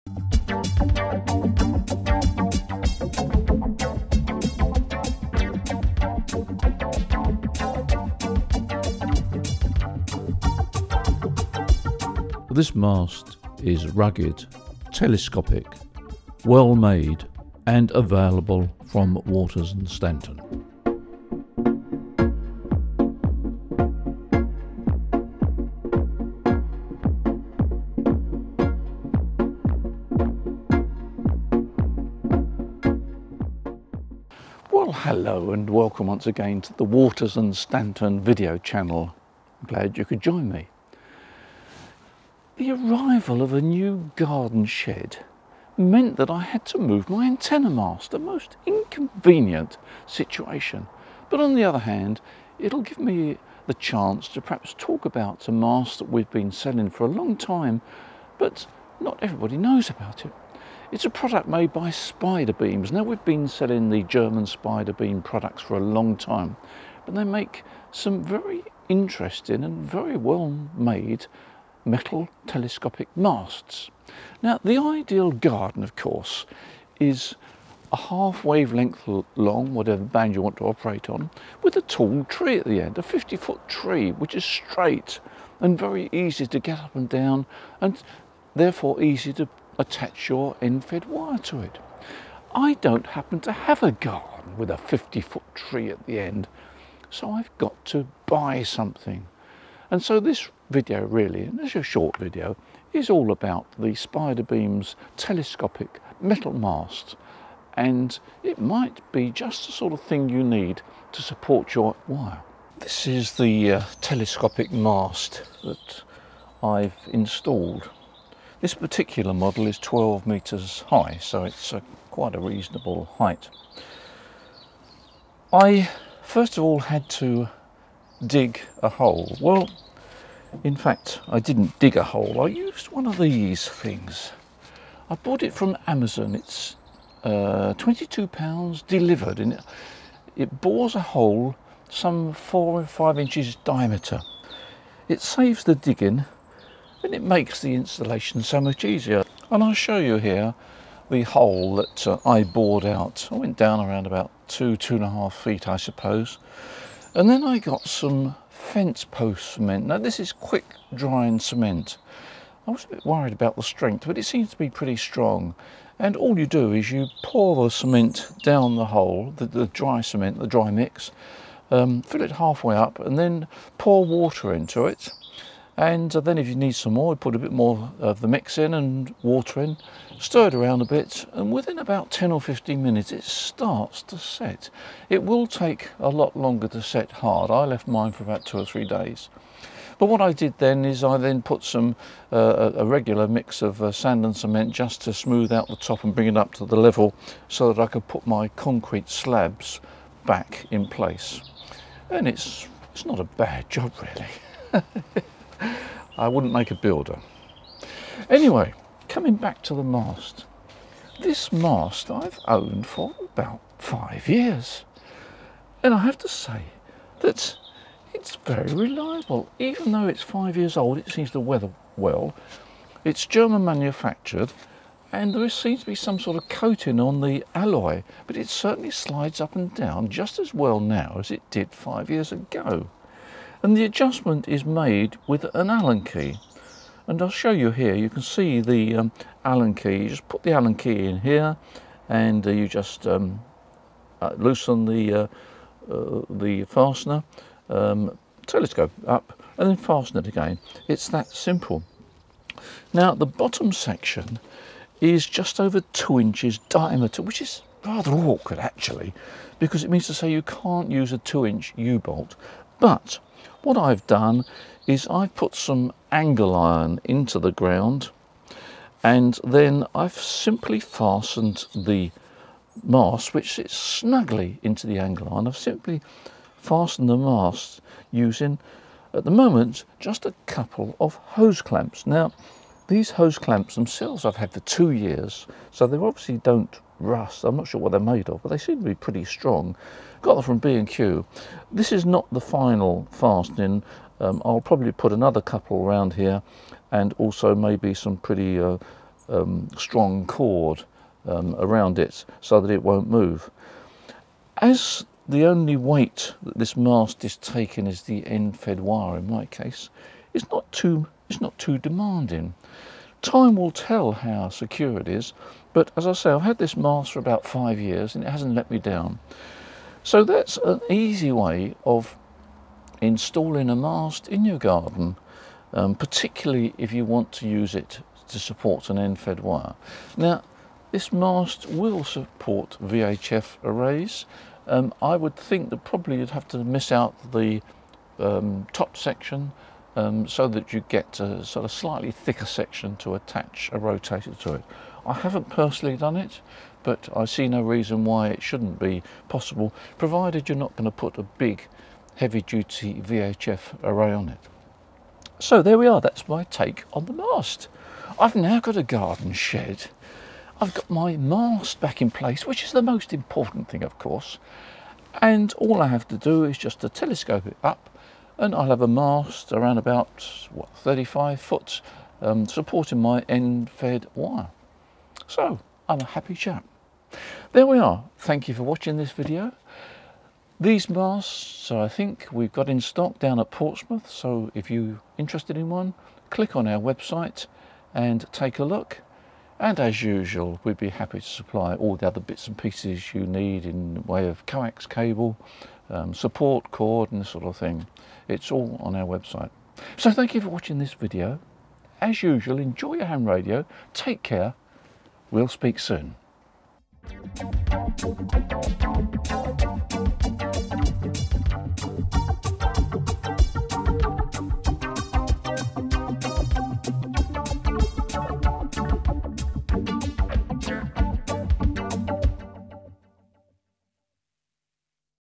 SIMPLE 10M SMALL GARDEN MAST - SPIDERMAST
Most if us need an antenna mast at some time. Here is a small garden telescopic solution for supporting a wire antenna.